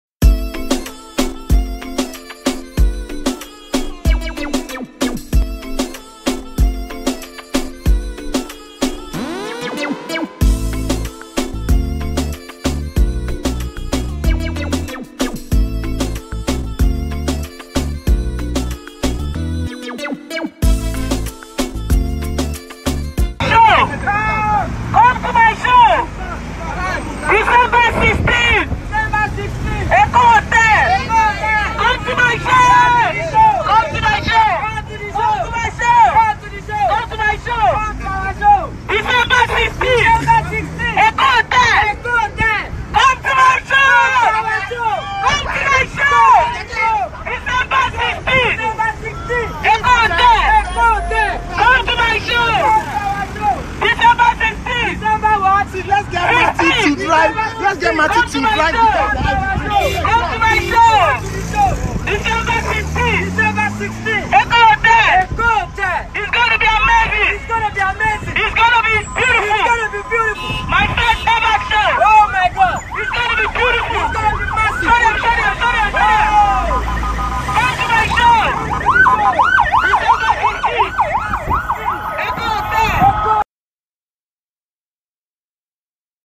Teni was in Lagos disguised as Santa Claus
Teni disguised herself as Santa Klaus for a special event